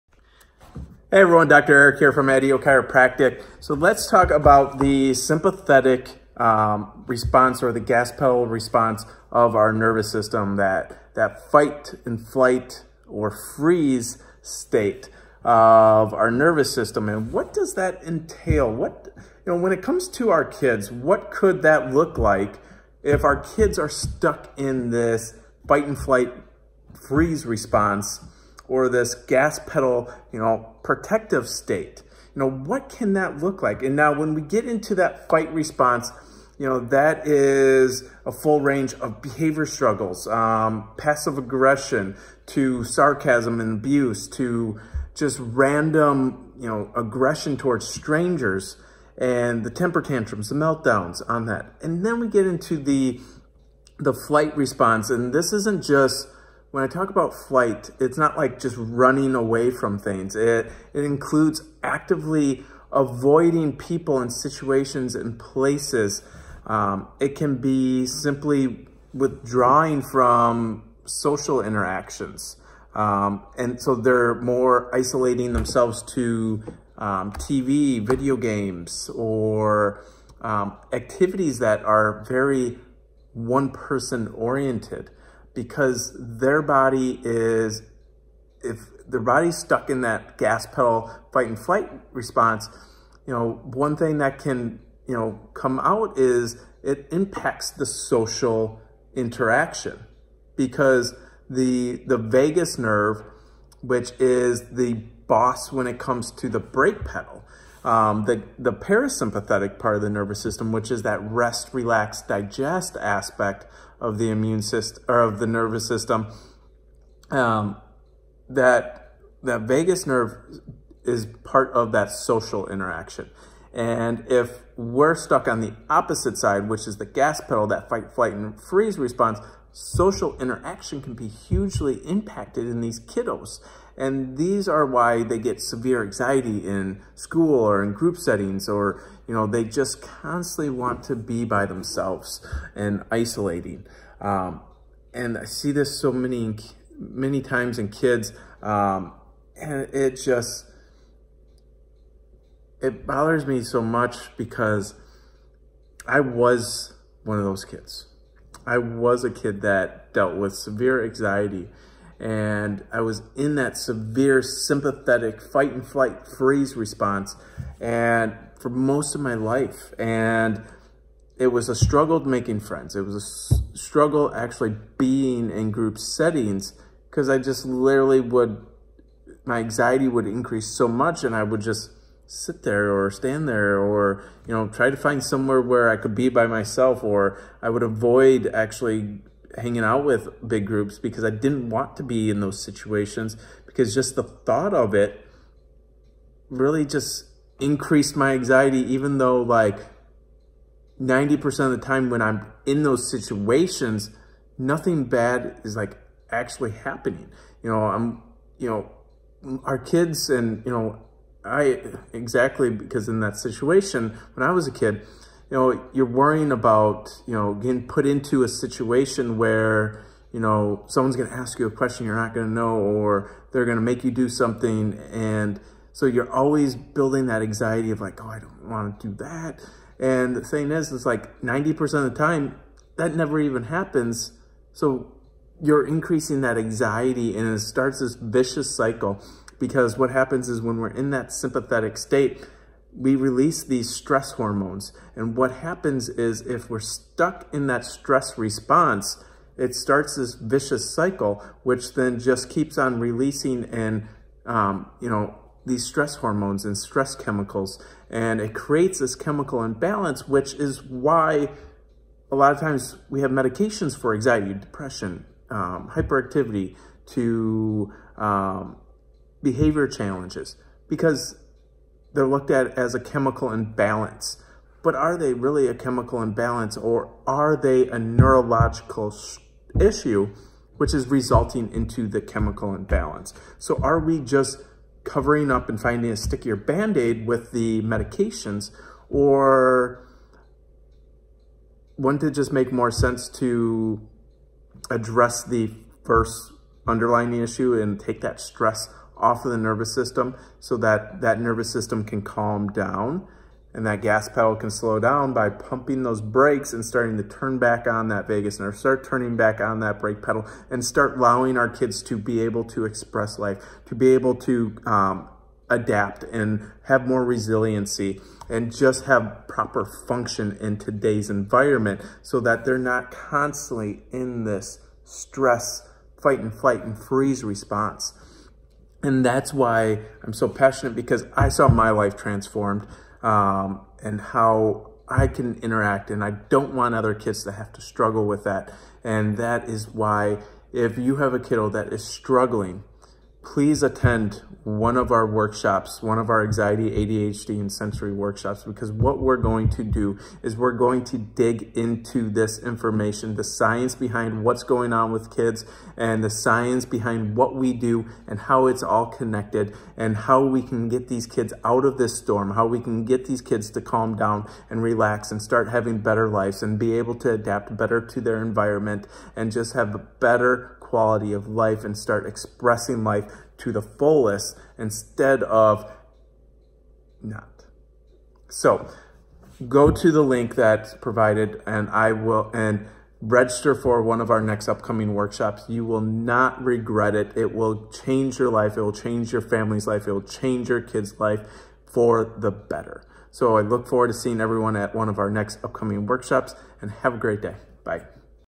Fight 💪 or Flight ✈
These are great responses when there is a bear chasing your kiddo, but what about living their everyday life while stuck in the sympathetic response?